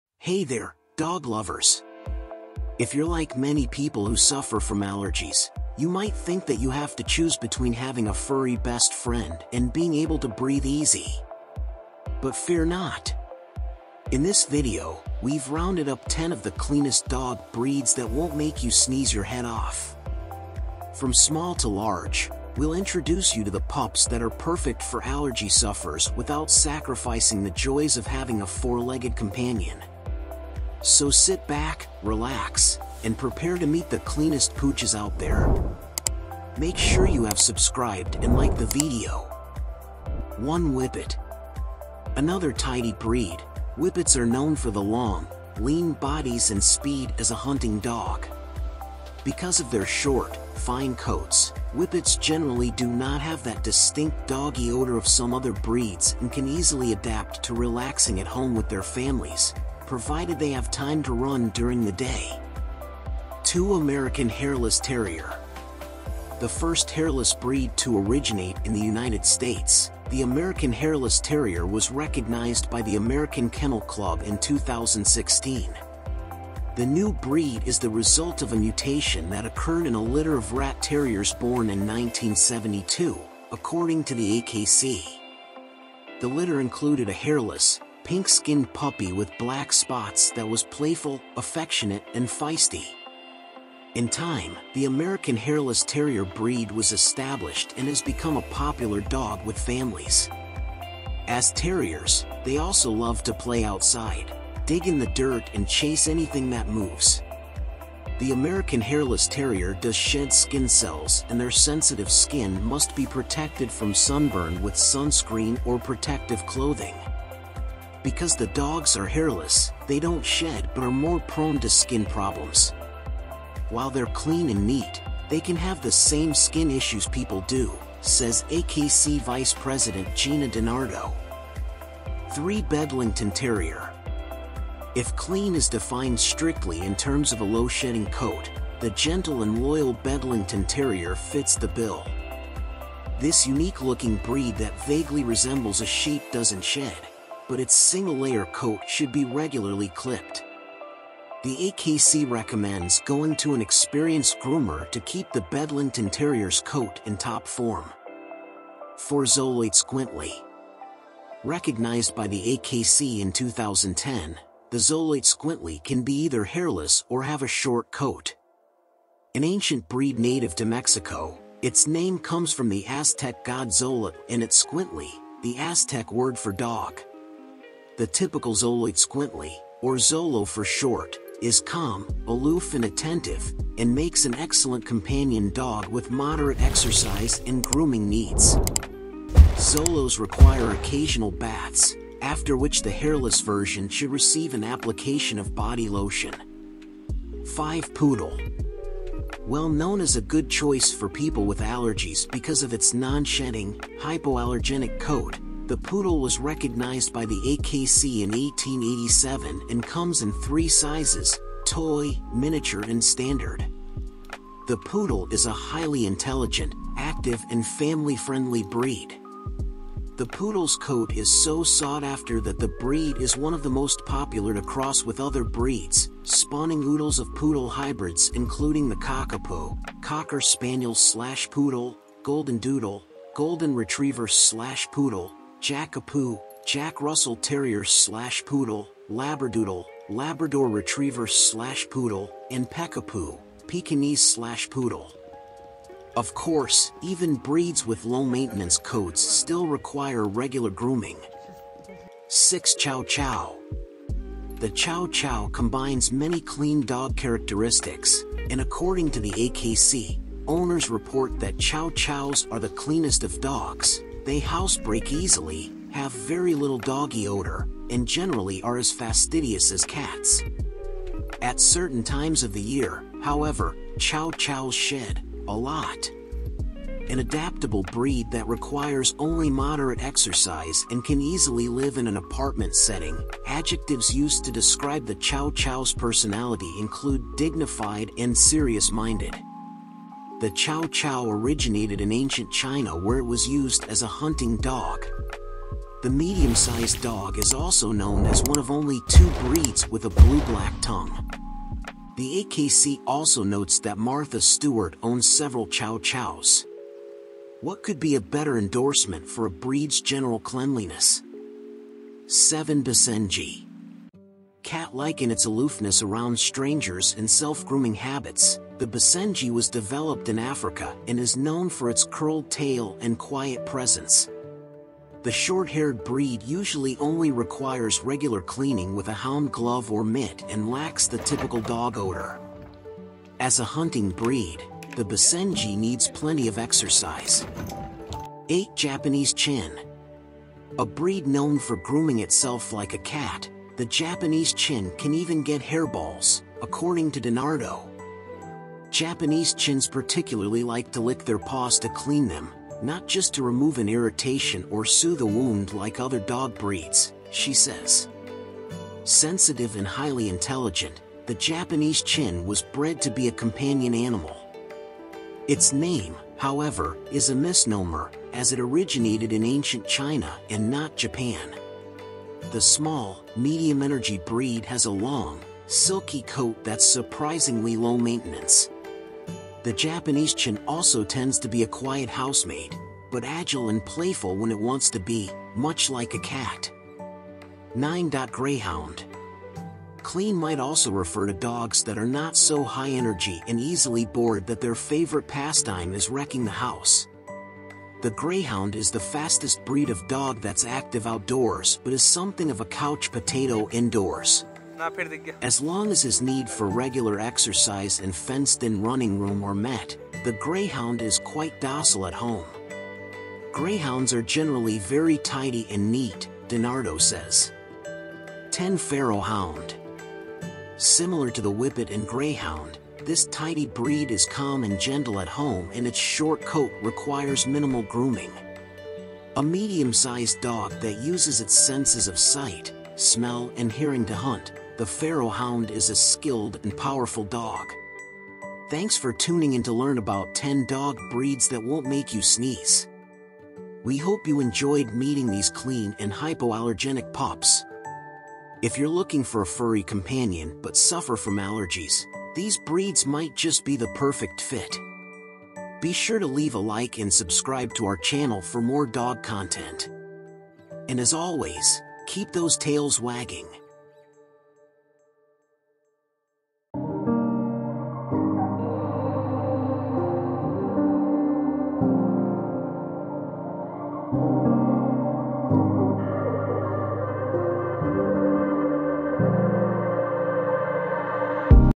the top 10 cleanest dog breeds that are perfect for neat freaks
Welcome to our latest YouTube video! In this video, we'll be discussing the Top 10 Cleanest Dog Breeds that are perfect for all the neat freaks out there. As much as we love our furry friends, we all know that taking care of them can sometimes lead to a messy home. But fear not, because these 10 dog breeds are known to be low shedders, easy to groom and always smell fresh.

Our list includes the Bichon Frise, Poodle, West Highland White Terrier, Basenji, Samoyed, Maltese, Chinese Crested, Portuguese Water Dog, Shih Tzu and the Irish Water Spaniel. We will also discuss each breed's personality, temperament, and activity level so that you can choose the best fit for you and your family.

Whether you're looking for an apartment-friendly dog or a companion for your family, these clean breeds have got you covered. So, if you're a neat freak or just looking for a low-maintenance furry friend, this video is a must-watch for you!
Our videos cover a wide range of topics from fascinating animal behavior and scientific discoveries, to fun and heartwarming animal videos.